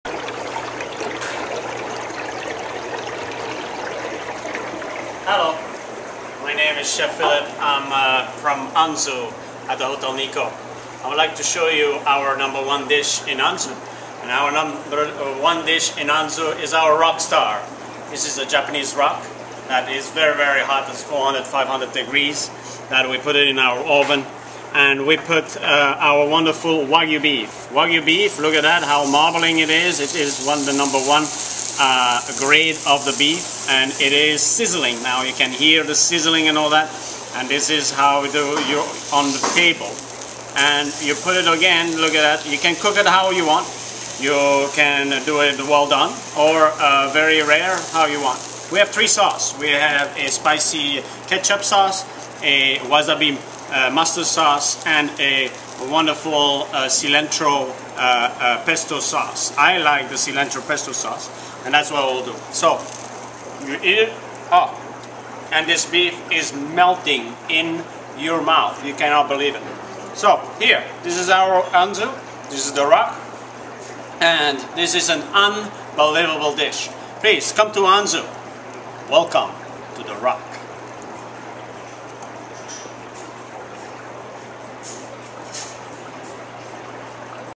The Rock Appetizer in ANZU with Chef Philippe
The Rock Appetizer in Restaurant ANZU at the Hotel Nikko in San Francisco, CA  introduced by Chef Philippe Striffeler